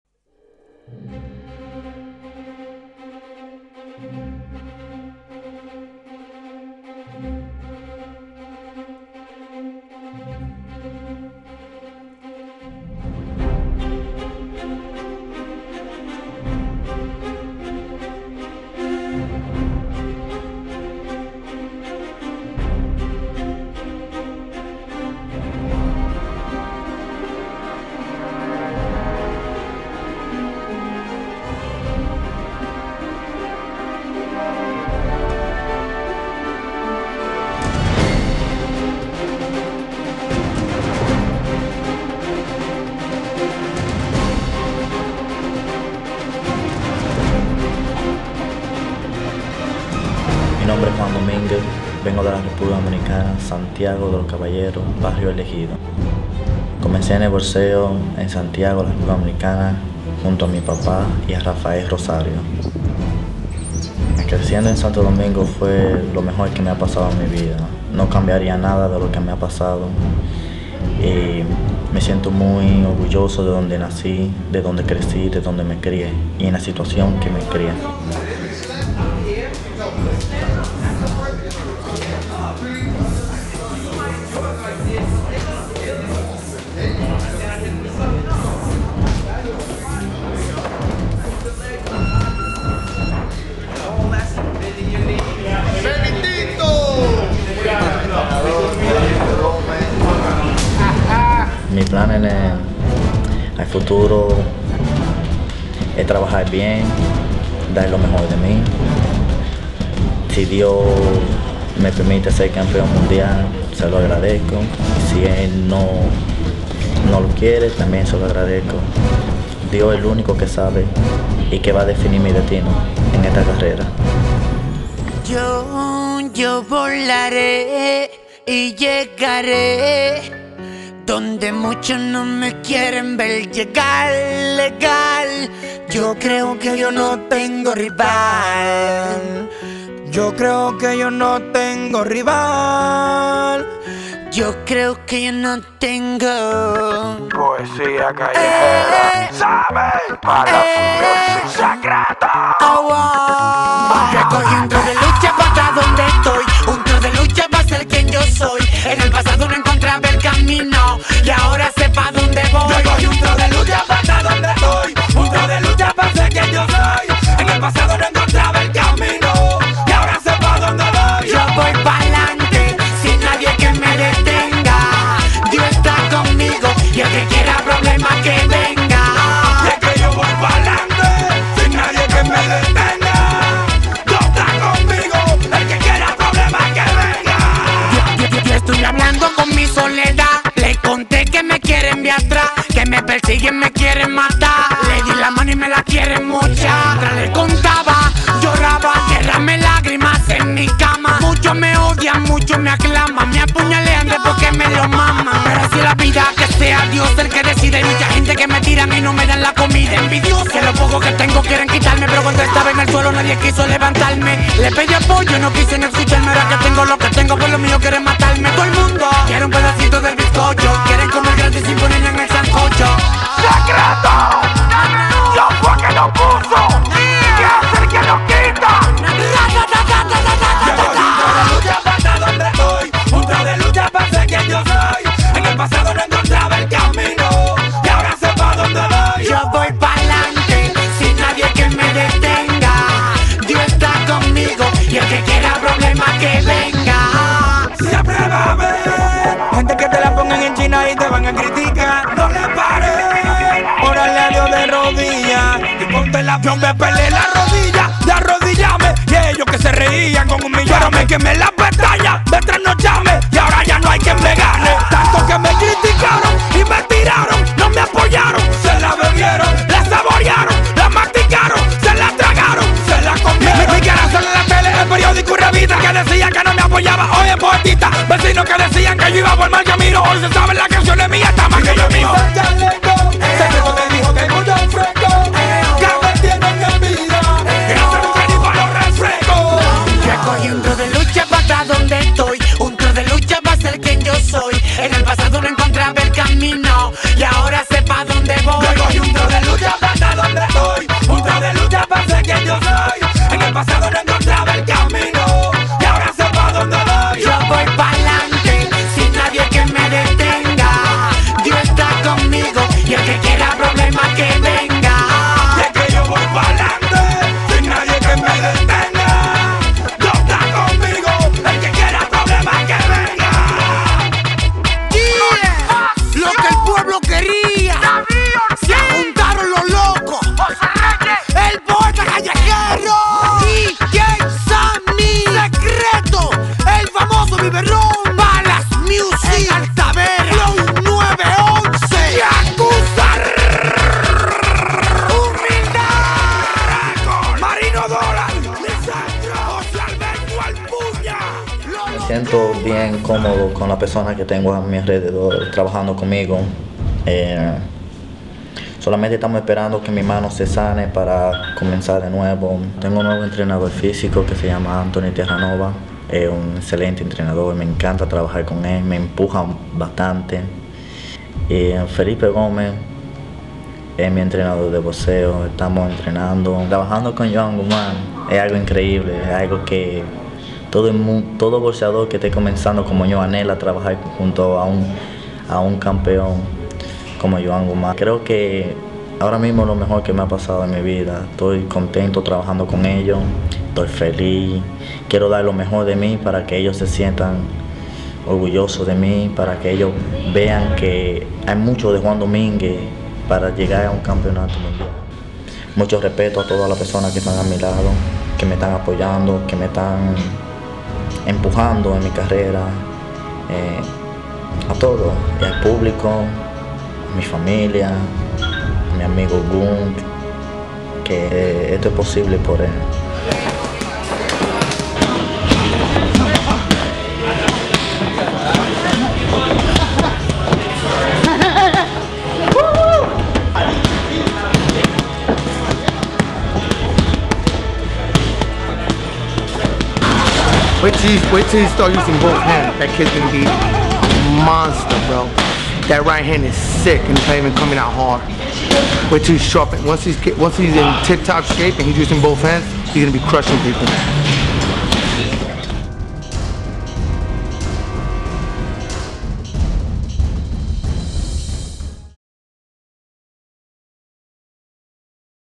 Baby Tito Webisode 1: The Introduction
A unique look into the life of undefeated Dominican boxing sensation Juan "Baby Tito" Dominguez.  First of many Webisodes.

 8-0 (5KO's)
New York State Bantamweight Champion